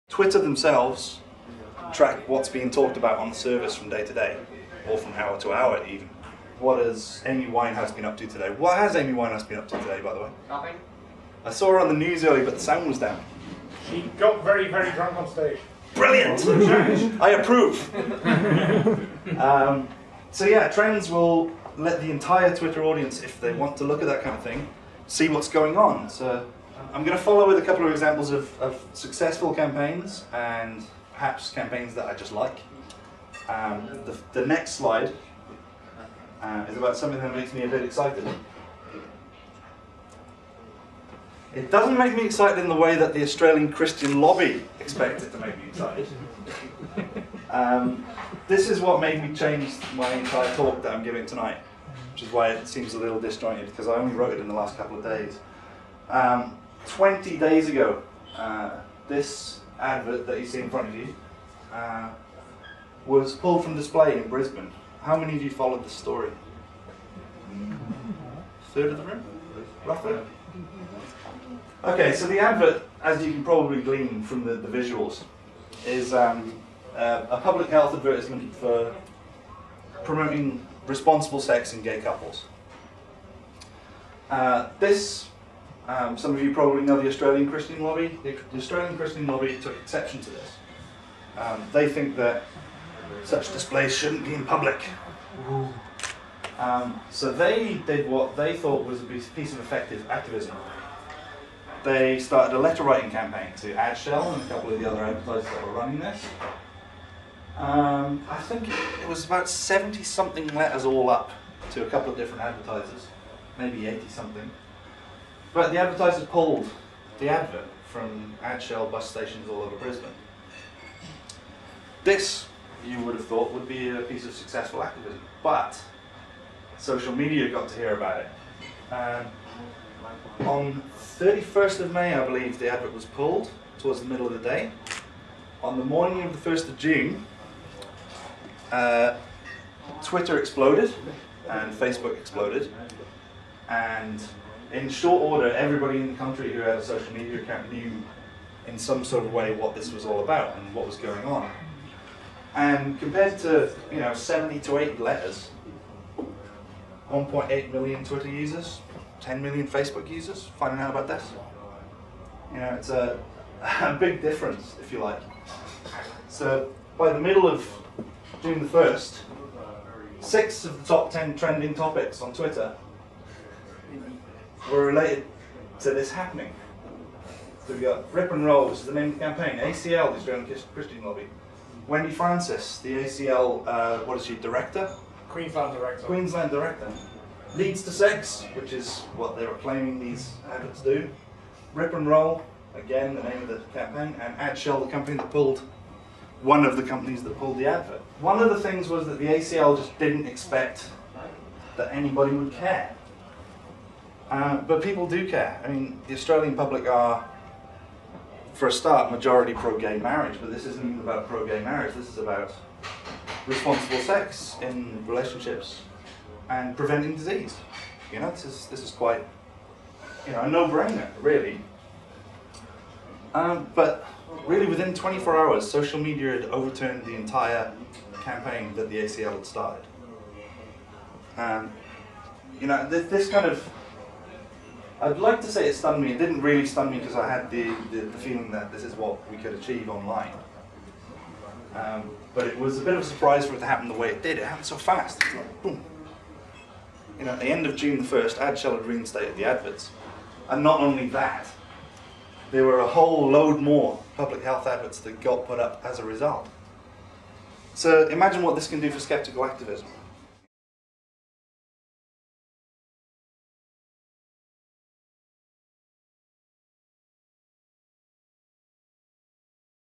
Jason Brown - Online Activism 1
Jason Brown talks about the Australian Christian Lobby's opposition to the Rip n' Roll safe sex campaign, and the social media response.